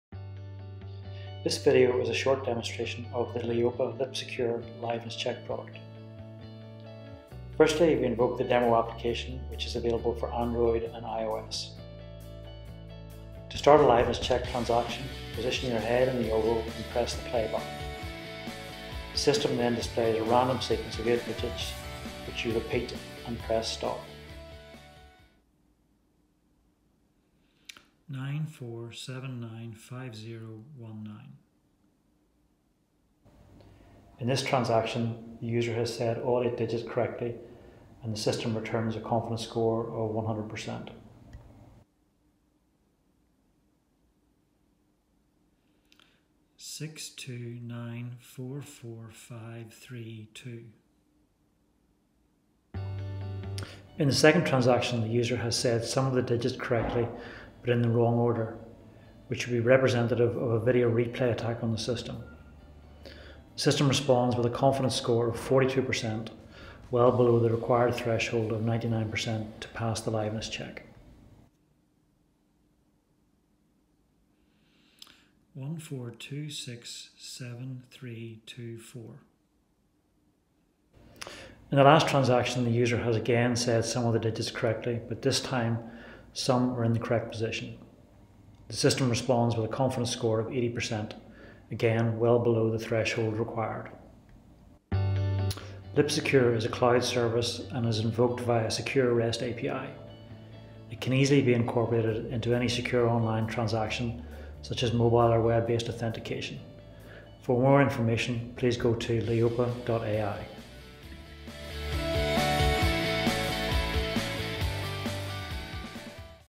LipSecure Video
This video is a demonstration of the Liopa LipSecure liveness check product. LipSecure can be incorporated into any secure online transactions, such as biometric authentication, to ensure that a live person is actually present.